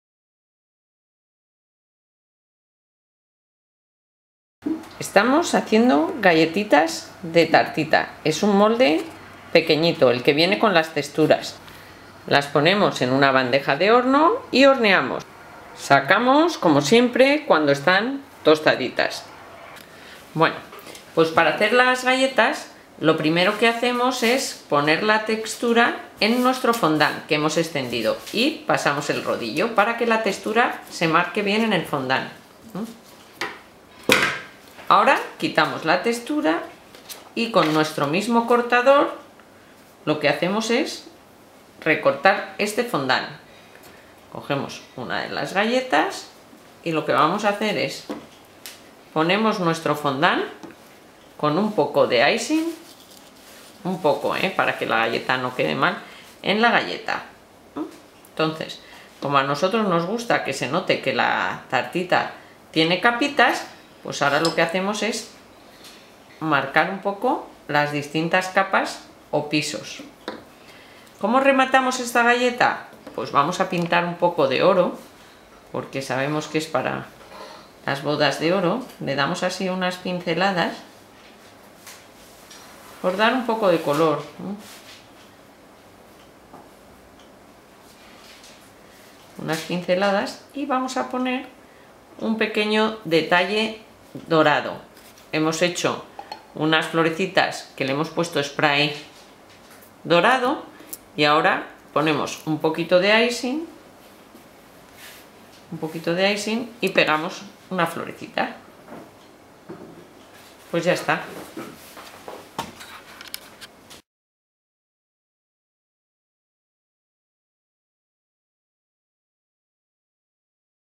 Curso de cookies. Galletas de tarta de boda. Cookies decoradas
En Keyks realizamos muchos cursos de cookies. Tenemos cursos a distancia y cursos presenciales . En este caso realizamos esta galleta cookie con forma de tarta para dar como recuerdo en una boda. Es un dulce regalo para los invitados que se hace muy rápido.

Una vez realizada la galleta le pusimos fondant blanco, la pintamos con colorante y le añadimos una pequeña florecita

Realizamos cursos presenciales y cursos a distancia.